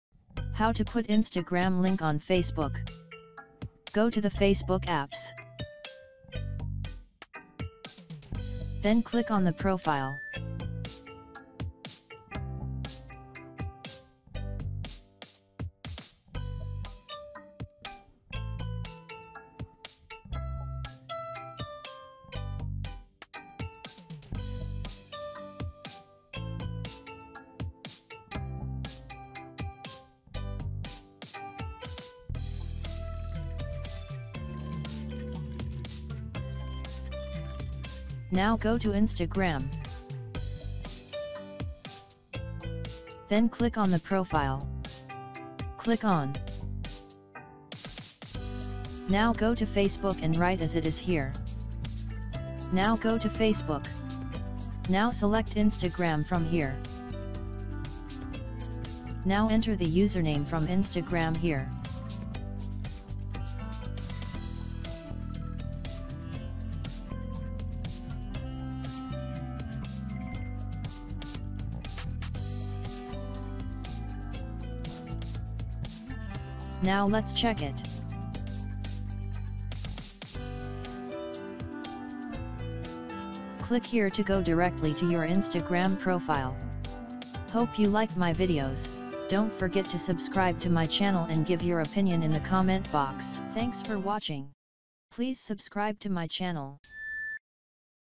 How to add Instagram link to Facebook
How to add the link on Facebook

How to add an Instagram link to Facebook
Go to the Facebook apps
Then click on the profile
Now go to Instagram
Now go to Facebook and write as it is here
 now go to Facebook
Now select Instagram from here
Now enter the username from Instagram here
Click here to go directly to your Instagram profile